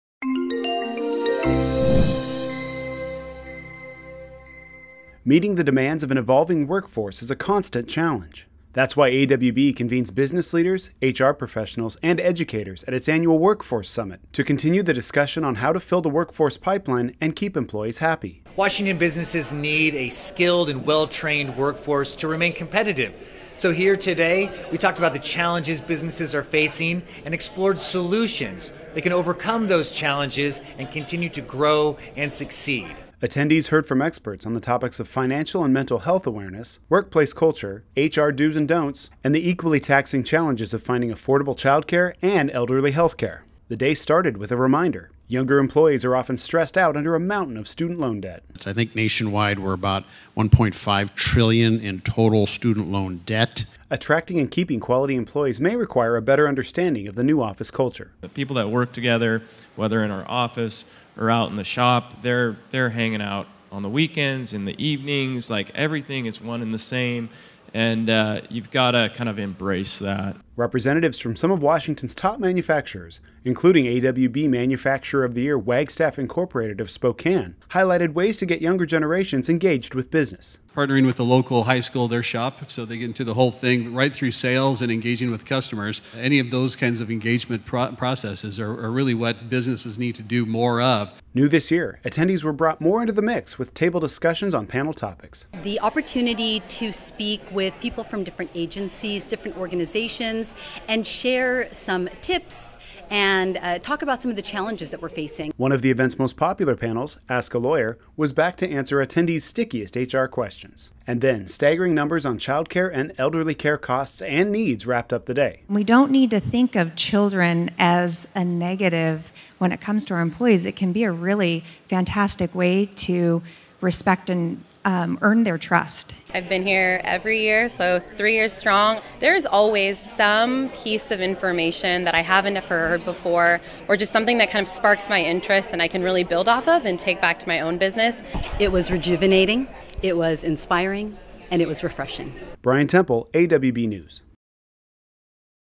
AWB News: 2019 Workforce Summit
AWB 2019 Workforce Summit continued to tackle the challenge of filling the workforce pipeline and keeping employees happy. This year's theme focused on employee culture. Please enjoy this look back at day full of useful tips and networking. @2019 Association of Washington Business